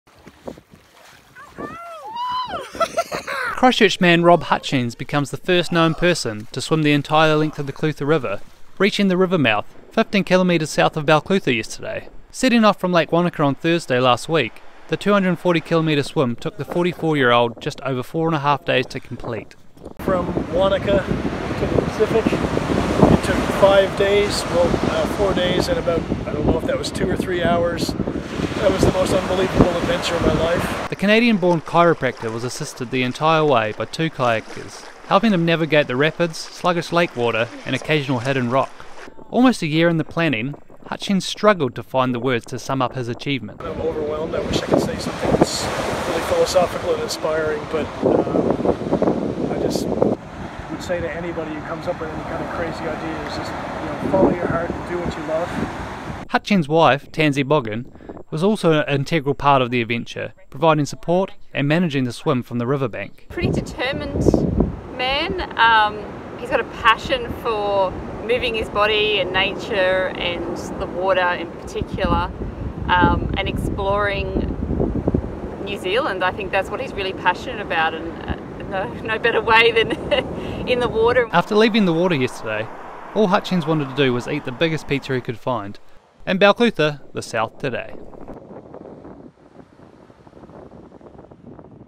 Man swims entire length of Clutha River
Dunedin's own, and only, local television channel.